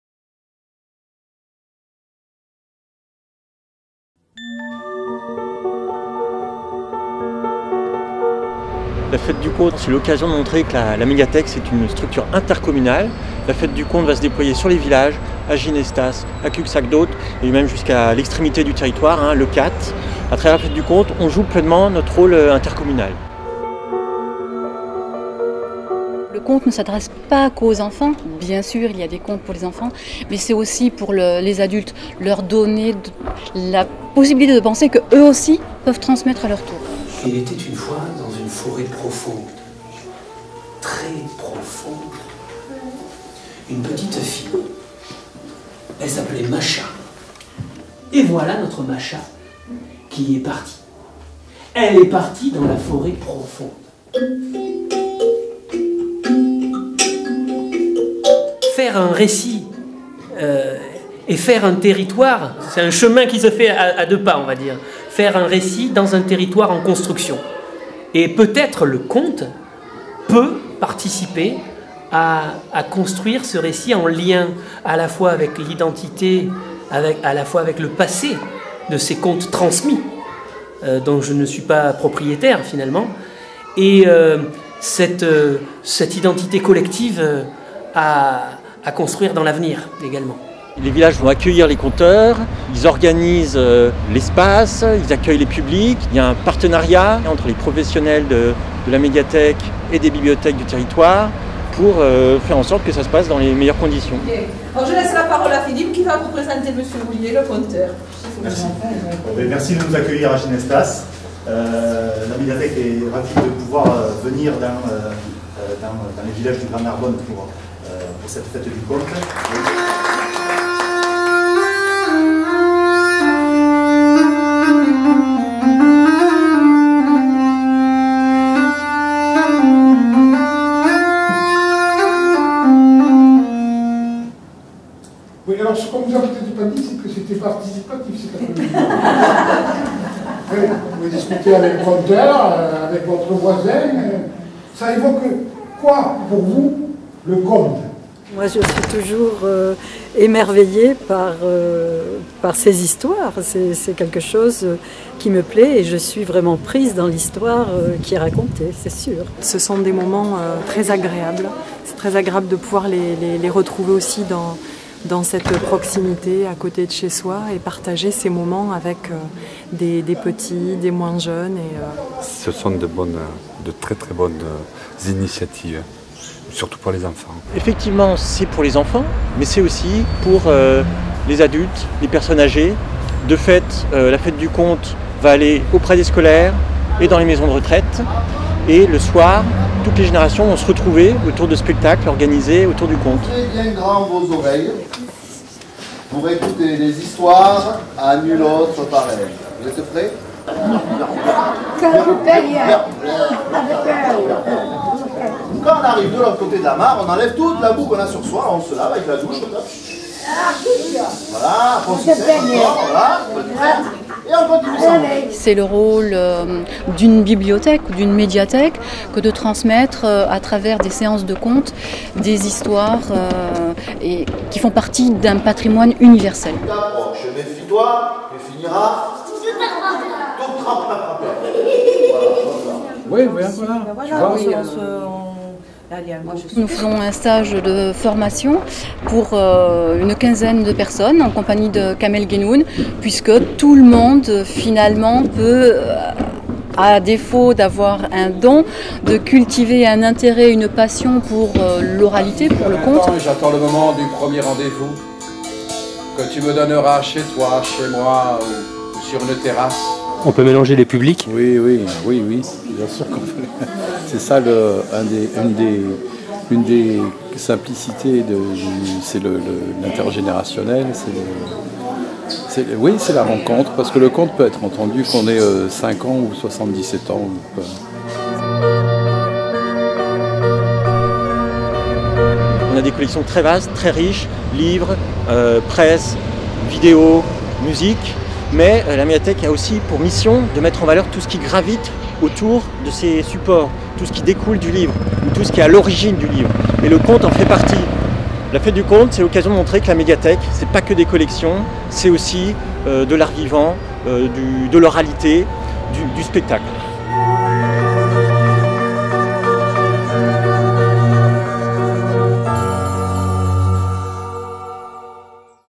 La Fête du Conte 2012 du Grand Narbonne
Pour sa 11ème édition, la Fête du Conte s'installe pendant trois jours dans Le Grand Narbonne, plaçant le conte sous le signe de la découverte et du partage en famille.

Une nouveauté de taille cette année ! Exceptionnellement, pour l'édition 2012 de la Fête du Conte, les personnes âgées de la résidence J.Séguéla de Salles- d'Aude se verront conter des histoires par élèves et conteurs au sein d'une assemblée intergénérationnelle, comme au temps des veillées. Les scolaires y sont invités pour réunir de multiples générations autour du conte.

Formez-vous à l'art du conte
Pour passer du « raconter » au « conter », la Médiathèque ouvre ses portes aux conteurs et stagiaires dès le mardi 13 novembre. Deux journées d'initiation où l'imaginaire, conté par Kamel Guennoun, envahira le public comme les lieux. Formateur, il permettra aux apprentis conteurs d'acquérir les outils pour dire des contes.

Paroles de talents
Vendredi 16 novembre, à 20h30, se tiendra une soirée consacrée aux talentueuses découvertes de la Médiathèque accompagnées de quatre conteurs. Le tout dans une ambiance familiale avec buffet dînatoire. Auditorium Jean Eustache - Médiathèque du Grand Narbonne.

Des contes dans les communes du Grand Narbonne
Samedi, tout l'après-midi, le public pourra partager un moment particulier en famille ou entre amis, dans sa ville ou son village, comblé de spectacles de qualité, enchanteurs. Un moment de partage de paroles et de culture. Les conteurs, seuls ou accompagnés de musiciens, vous conteront leur vision de la vie, leurs aspirations, leurs pensées.

Knup, conte-concert à partir de 6 ans
Samedi 17 novembre, à 17h, au Théâtre / Scène Nationale.
Dans ce spectacle mêlant avec délice et surprises le conte merveilleux et les musiques originales, Luigi Rignanese et son Quatuoraconte nous font voyager à travers les temps. Une épopée barbare à gorge déployée, peuplée de parents récalcitrants, de sirènes siliconées, d'éclairés illuminés, du Dragon d'Or et bien des pires encore... Une histoire de transmission sans courroie de sécurité, un conte-concert en CinémaScope sur l'urgence de secouer à pleine main son destin avant demain.

Paroles de conteurs
Samedi 17 novembre, à 20h30, une invitation au voyage avec K.Guennoun où le conte sera roi et la parole fantastique reine. Auditorium Jean Eustache - Médiathèque du Grand Narbonne.

Un dimanche à la Médiathèque...
pour partir à la découverte des conteurs et se laisser surprendre par leur talent d'improvisation au gré des histoires et devinettes qui se croisent. Ambiance festive pour toute la famille !

Tous les spectacles sont libres et gratuits dans la limite des places disponibles.